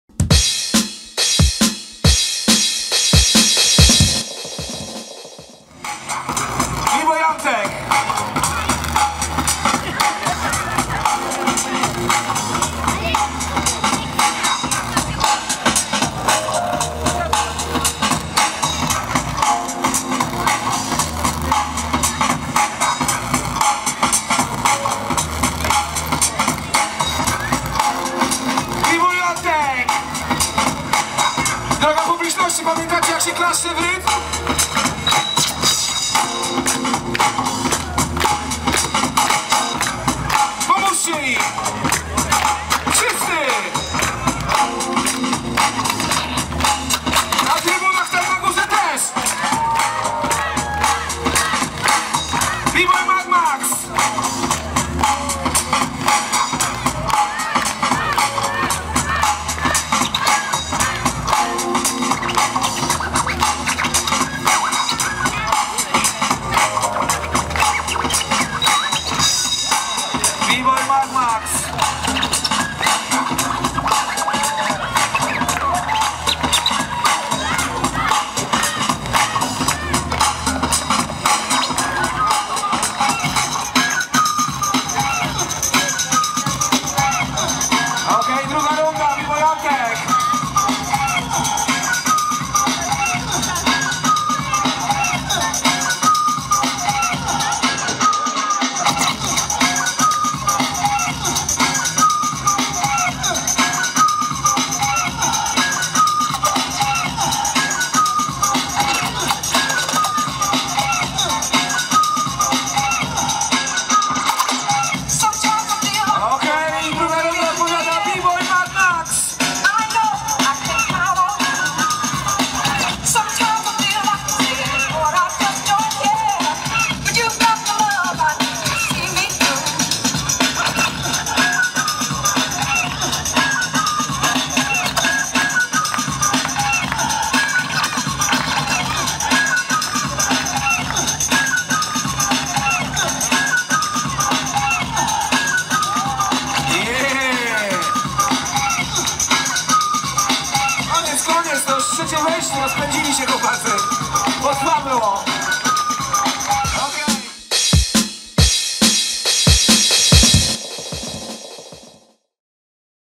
Brejkdans vol.3 / Finał Bboying Kids Battle 1vs1 / 12-14 lat / Antuan vs MadMax
Brejkdans vol.3 / Finał Bboying Kids Battle 1vs1 / 12-14 lat / Antuan vs MadMax / Piotrków Trybunalski - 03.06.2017.